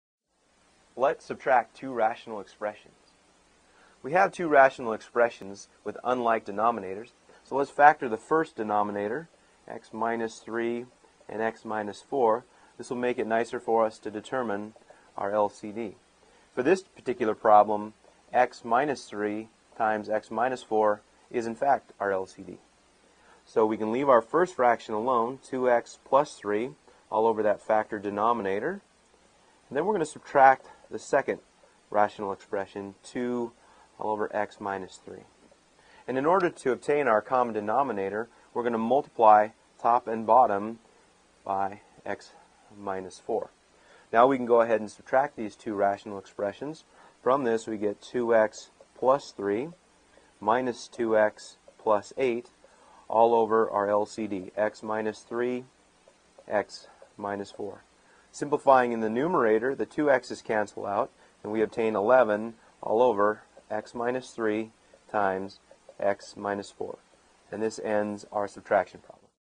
Chapter Test Prep – Ch P, Exercise 17
An instructor walks you step-by-step through the solution to Exercise 17 in the Chapter P Test from the textbook Blitzer Precalculus 7e.